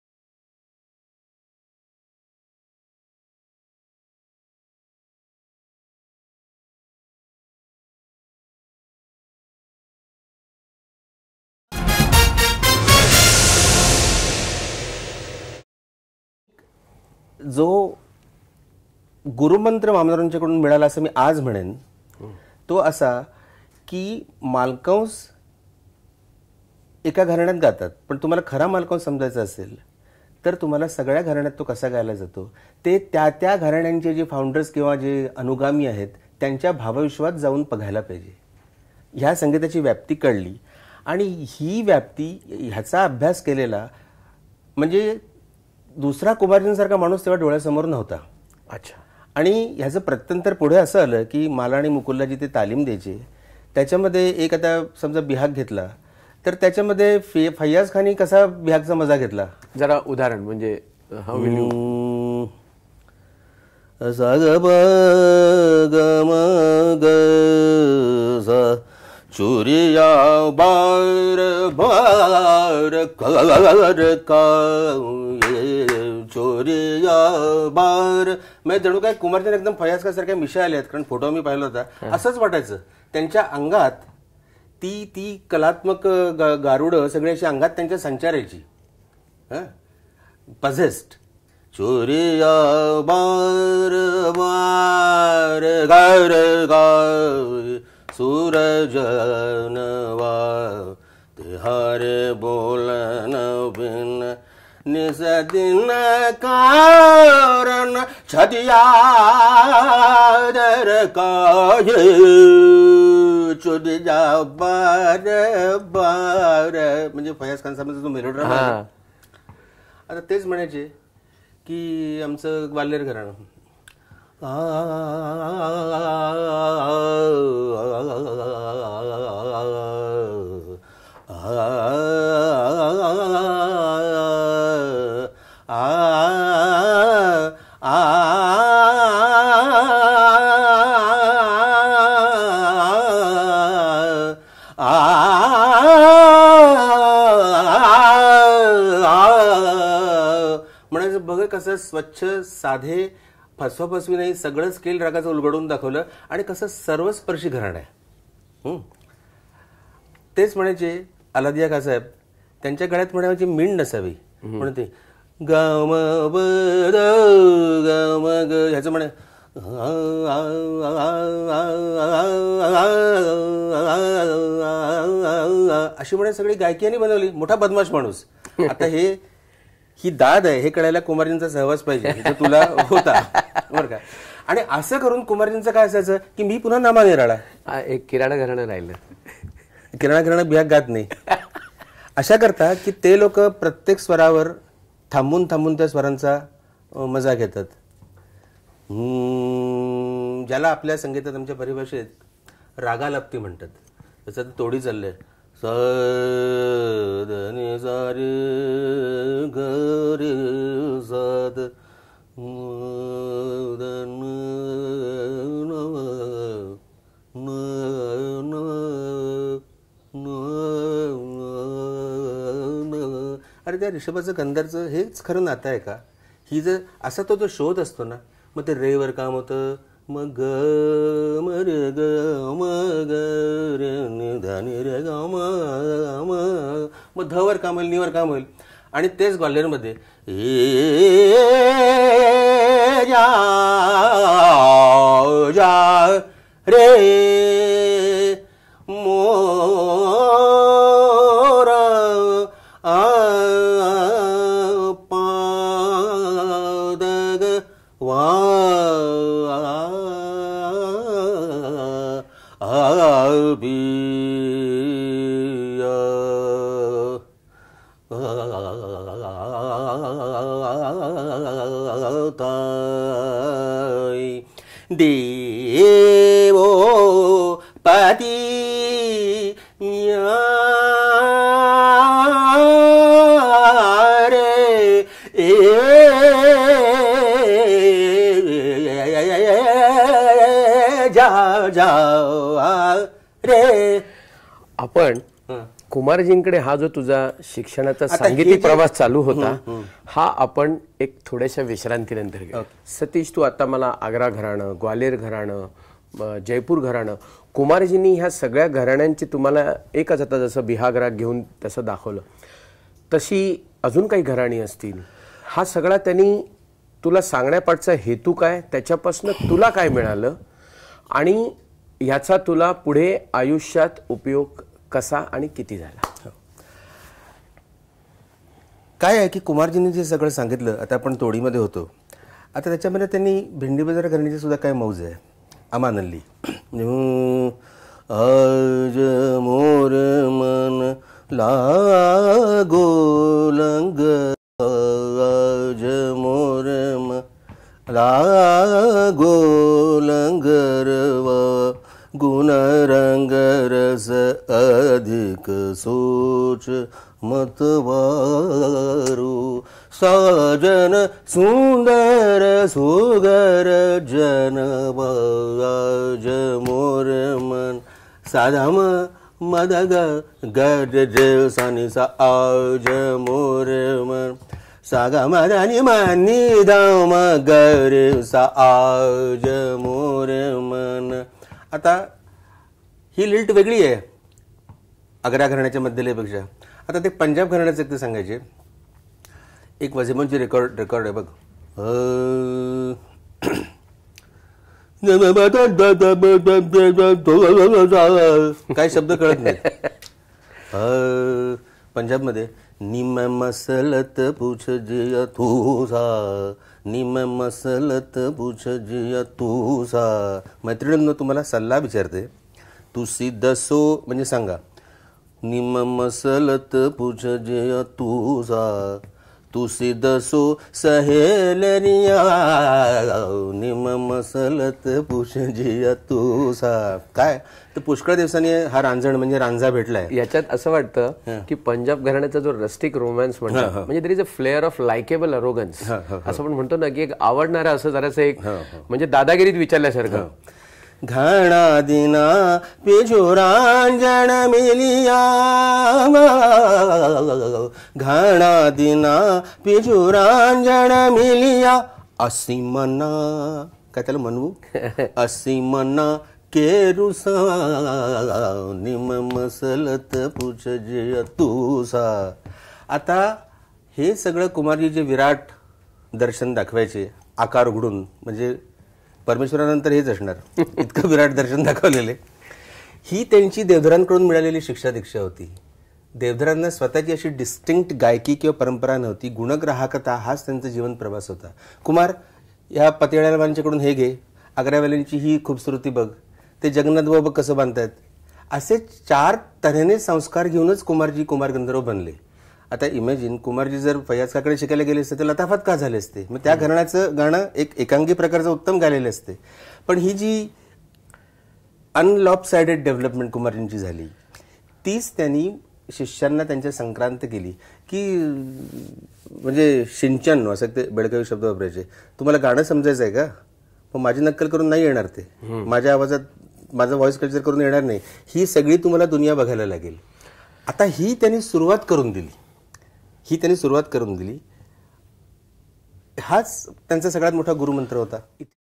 Pt. Kumar Gandharva's Taleem - Pt. Satyasheel Deshpande
Satyasheelji talks to Shashi Vyas about the education he received from Kumarji and how Kumarji exposed him to the vastness and diversity of Hindustani music.

A discussion on Hindustani (Indian) Classical music by Pandit Satyasheel Deshpande - foremost disciple of Pandit Kumar Gandharva. for more...